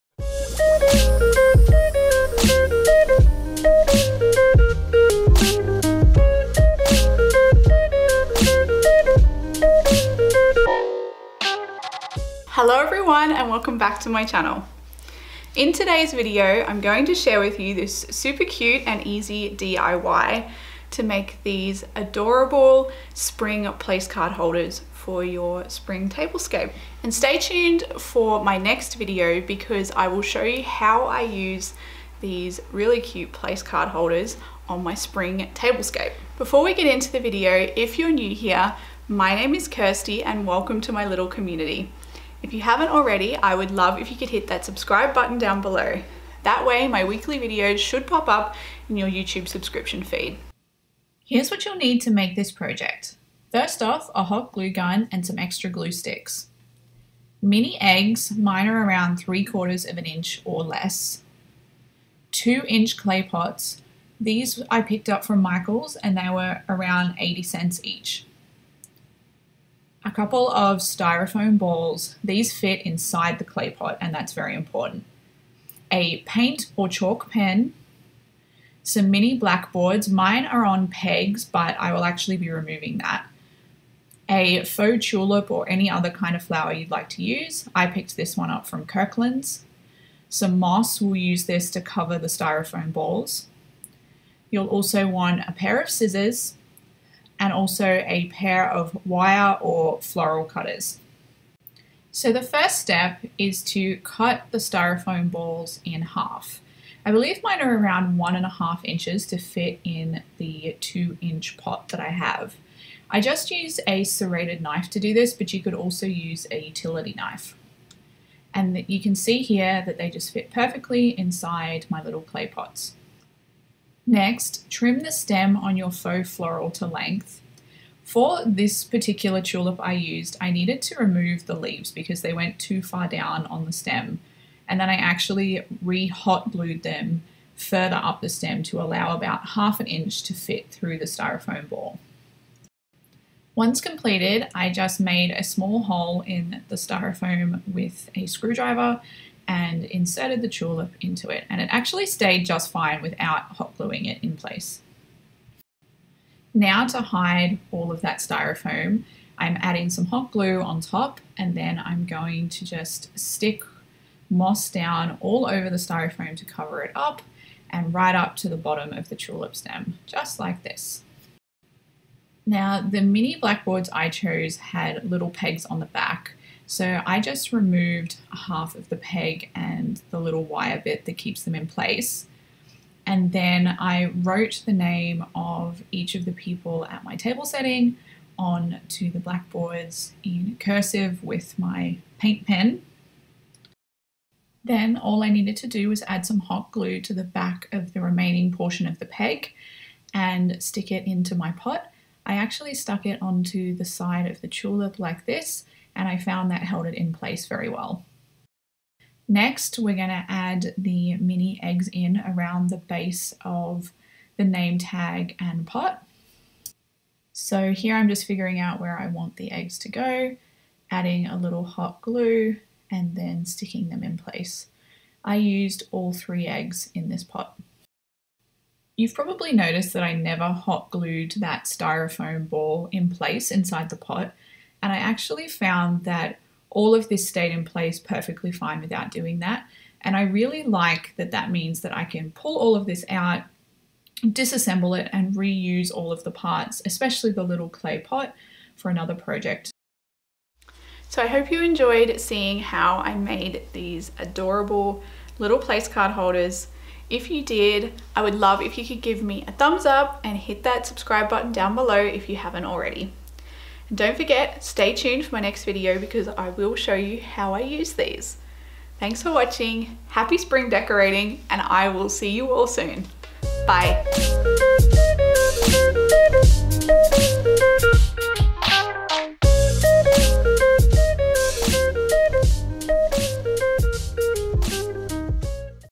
DIY SPRING PLACE-CARDS | Easy and Inexpensive | Farmhouse Table Do-it-Yourself
Spring is fast approaching and to kick off the DIY and decorating for the season, lets start with this easy and inexpensive DIY spring place-card. Made almost entirely with items from Michaels, this project will take less than 10 minutes to make!

This year I'm playing around the pops of yellow in my home. It's one of my favorite colors and is so cheery for Spring. I'm also trying to do more DIY projects for my home because they're not only cost saving but you can make exactly what you imagine!

Here is my blog post on this topic where you can find all of the product links I mentioned

Will you give this easy DIY a try in your own home? Share your thoughts with me in the comments below, I love to hear from you!

Until next time, happy decorating.

Kirstie